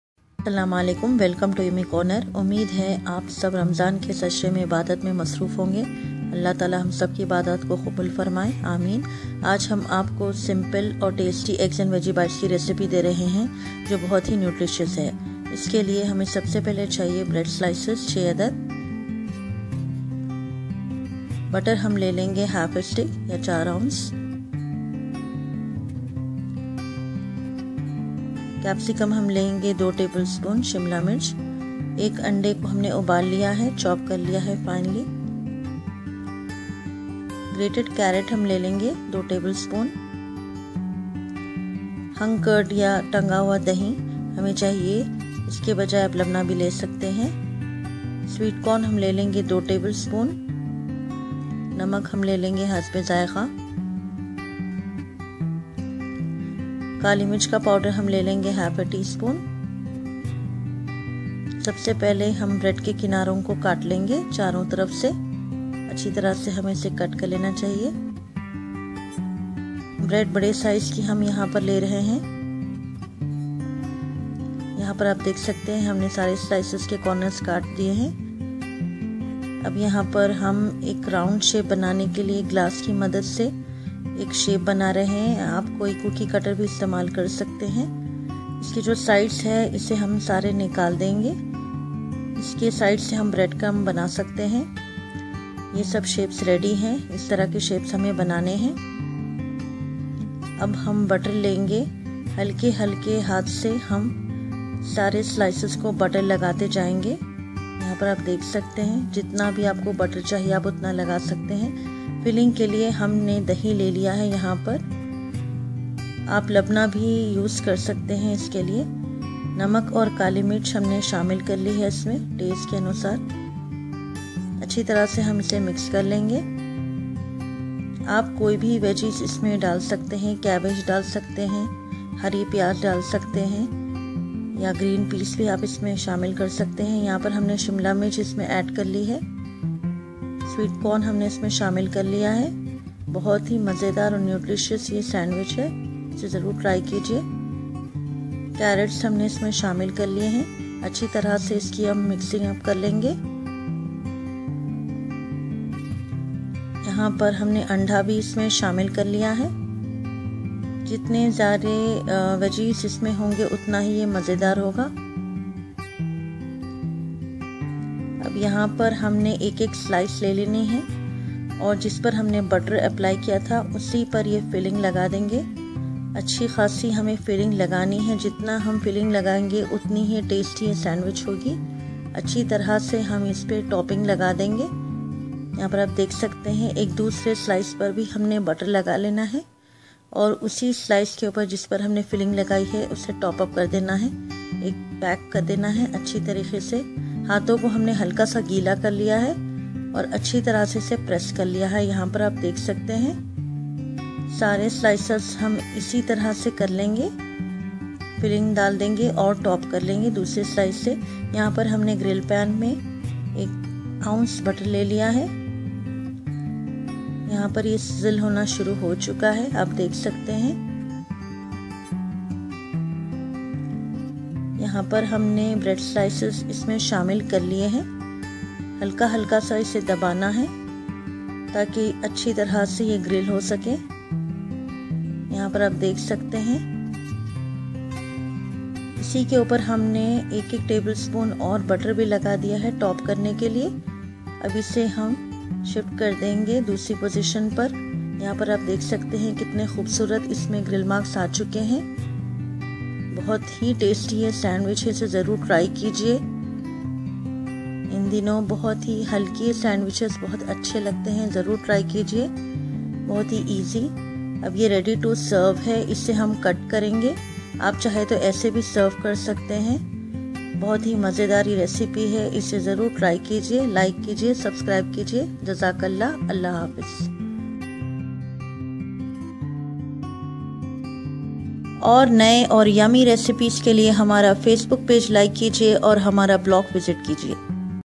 Egg and Veg Bites | Yummy Corner
Try these quick and nutritious sandwiches for your snacks!

Ingredients

Butter 1/2 stick
Chopped Green Bell Pepper 2 tbsp
1 boiled egg chopped
Grated Carrot 2 tbsp
Hung Curd or Labneh 1/2 cup